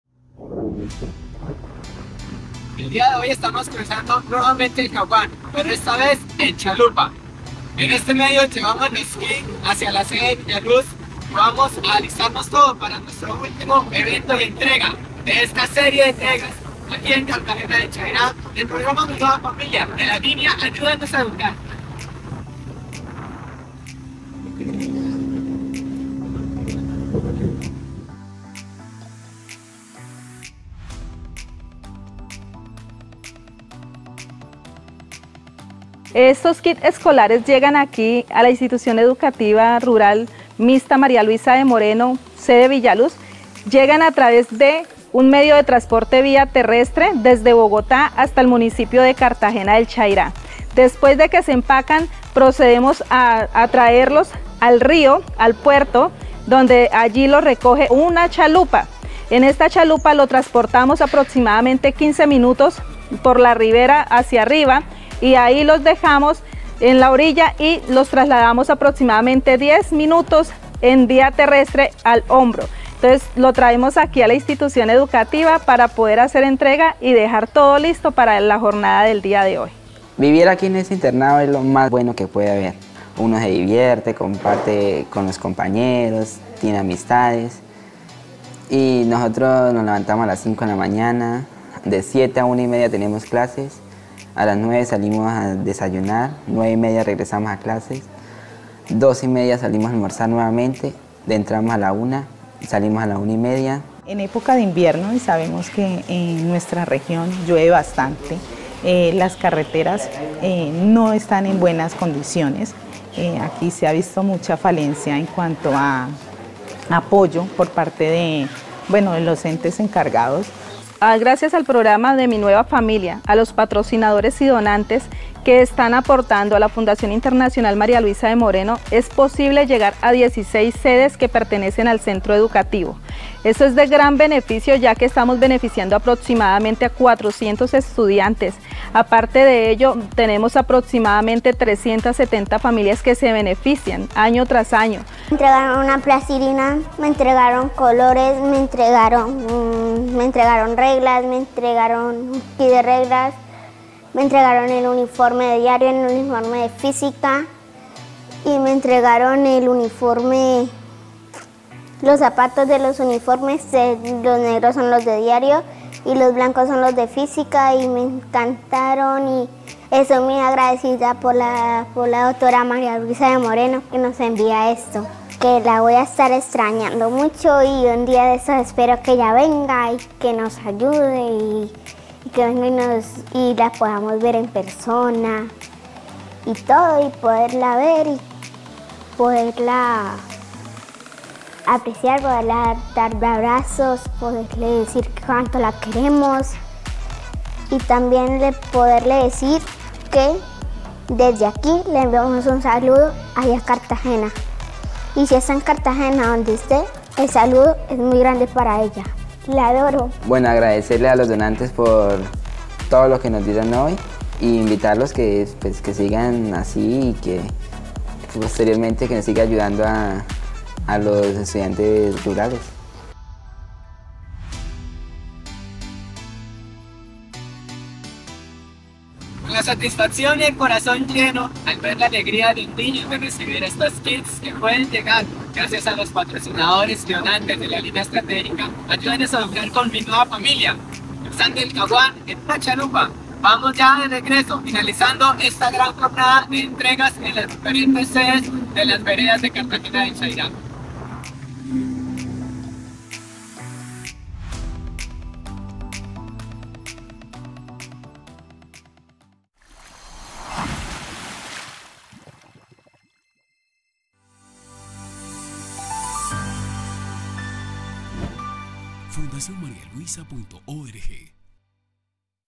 🇨🇴 | Parte 3 | Entrega de uniformes y kits escolares en Villa Luz, Caquetá
Hoy este viaje termina junto a los niños de Villa Luz 🇨🇴. En avión, camioneta y bongo llegamos hasta Cartagena del Chairá con el único objetivo de llevar felicidad y educación de calidad para los niños patrocinados por el programa MiNuevaFamilia en Caquetá.

📣Todos los domingos nuevo video | 🔔 Activa las notificaciones